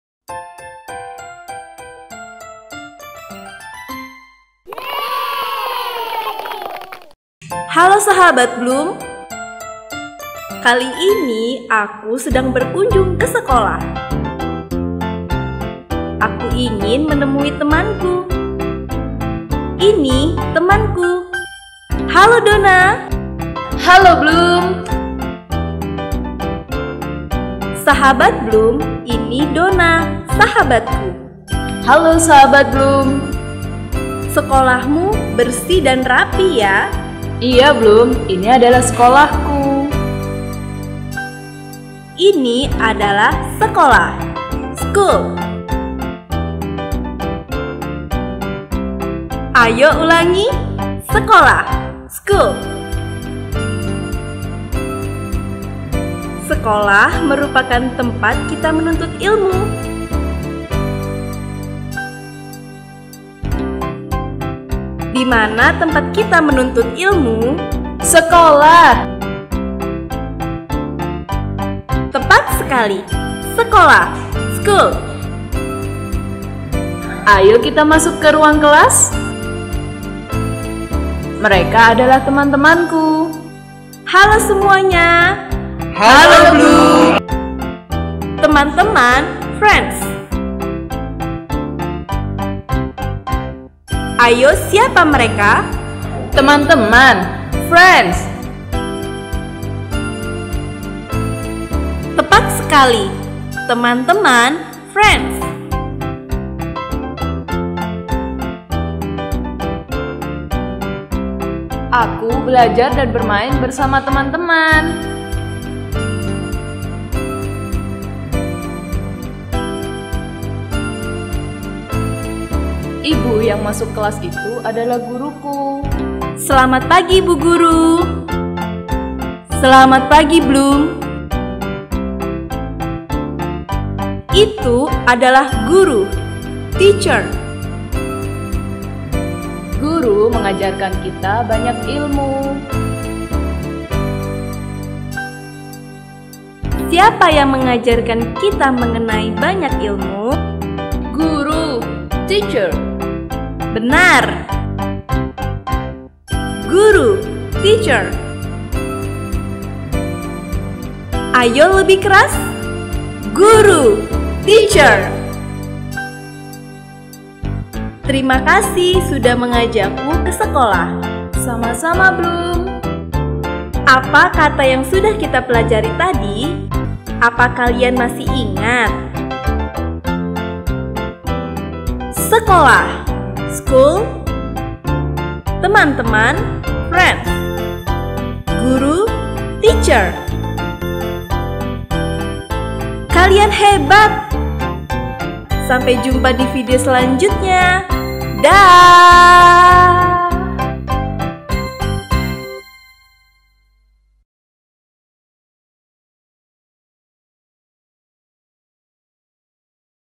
[Video Edukasi Anak] Pembelajaran Untuk Toddler : Kelasku - My Class
Hai adik-adik, dalam video kali ini Bloom akan mengajak kalian mengunjungi sekolah dan kelas bersama dengan Dona.

Yuk tonton videonya bersama dengan bermain sambil belajar.
Ingin dapatkan materi pembelajaran Toddler lainnya?